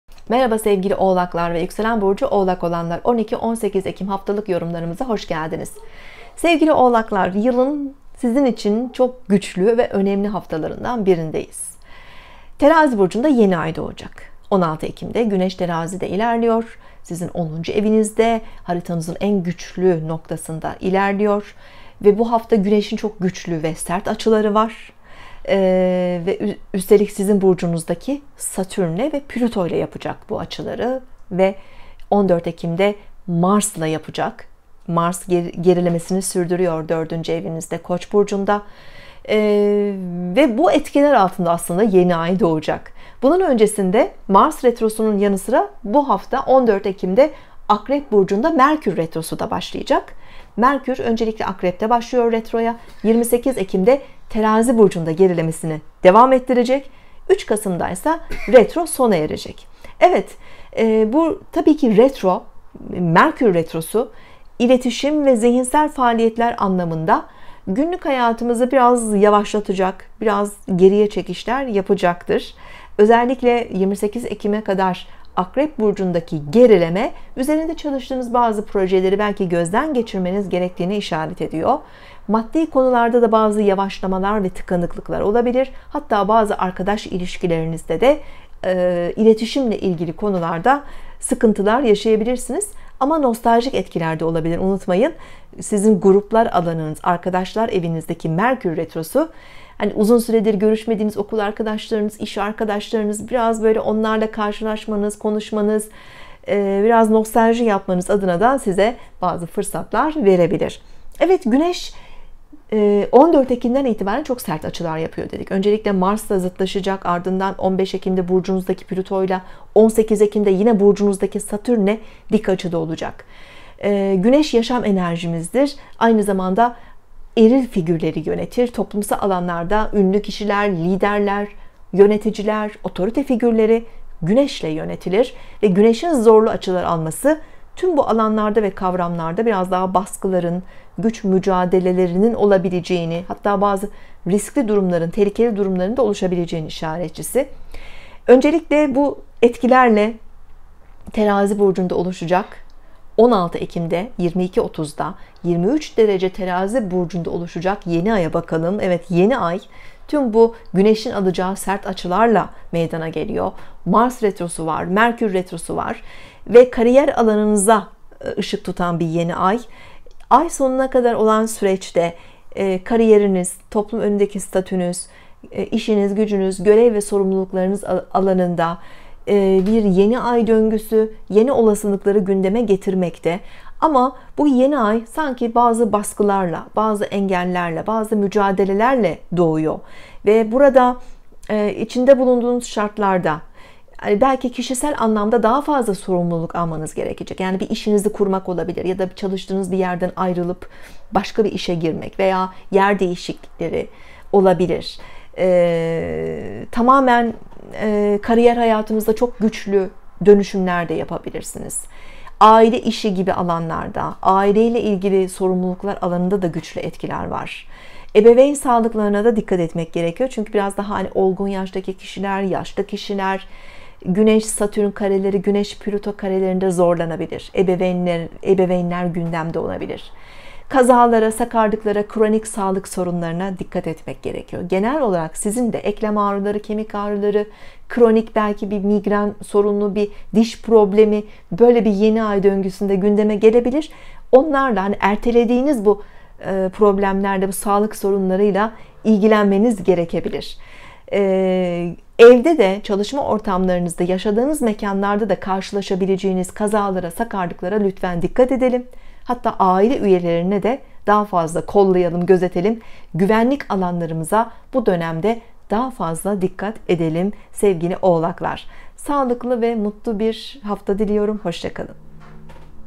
Oğlak Burcu, Haftalık Burç Yorumları, 12-18 Ekim 2020 (Astroloji)
12-18 Ekim 2020 Haftalık Burç Yorumları (Oğlak ve Yükselen Oğlak Burcu)
Oğlak ve Yükselen Oğlak Burcu 12-18 Ekim haftası haftalık sağlık, iş, kariyer haftalık burç yorumları (Astroloji)